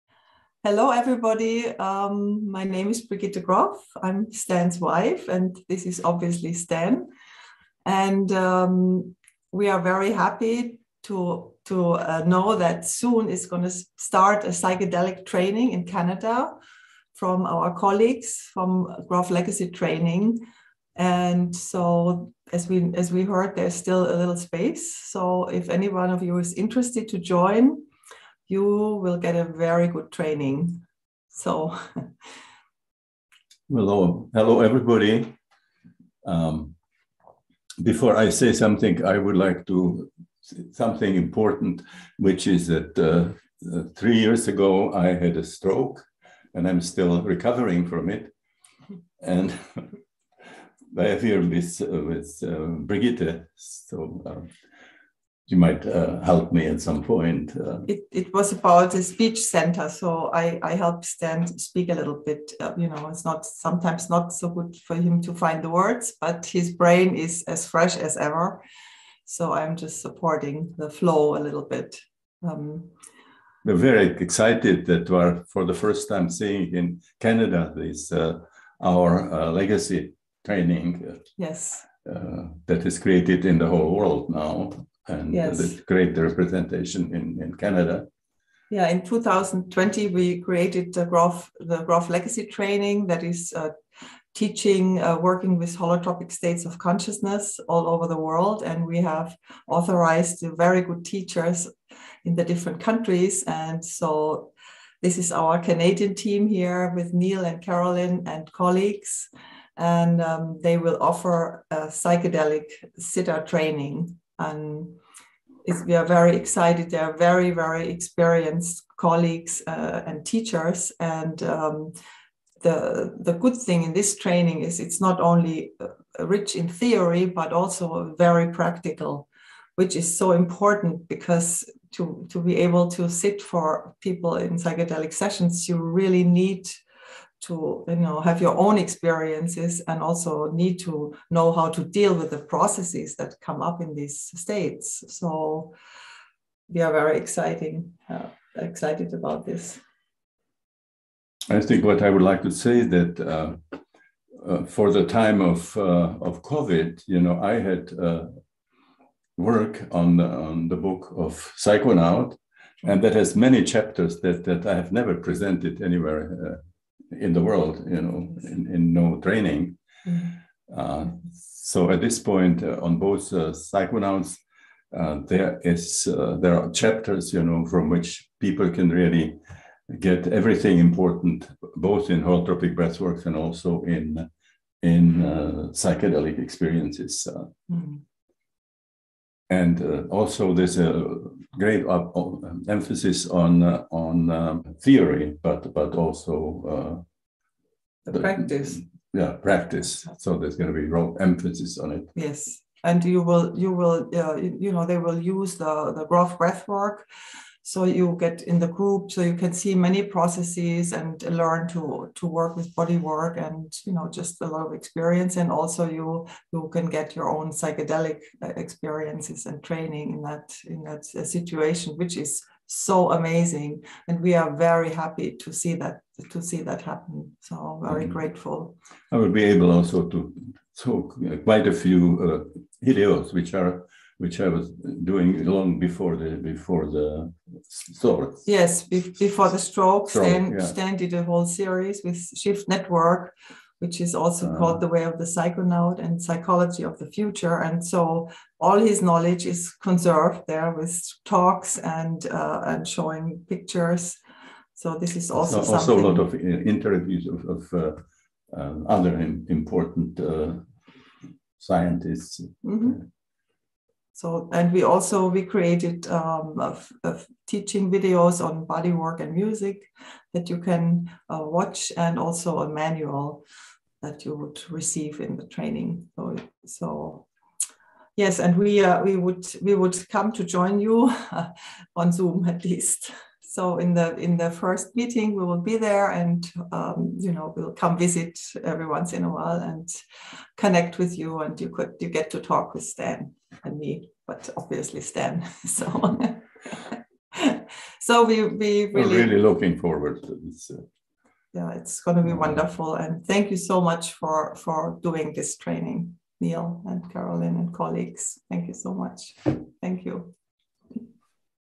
Announcement for the Canadian Grof® Legacy psychedelic sitter training 2022
Welcome to the launch of Grof® Legacy Training Canada, the Canadian part of Grof® Legacy International. Our first course offering begins June 1, 2022 with The Way of the Psychonaut Part A. There is still space available. We look forward to meeting and working with you! We are excited to have Stan and Brigitte Grof in attendance for the first Zoom call of the course, June 5, 2022. Register now for this great training opportunity...our store is open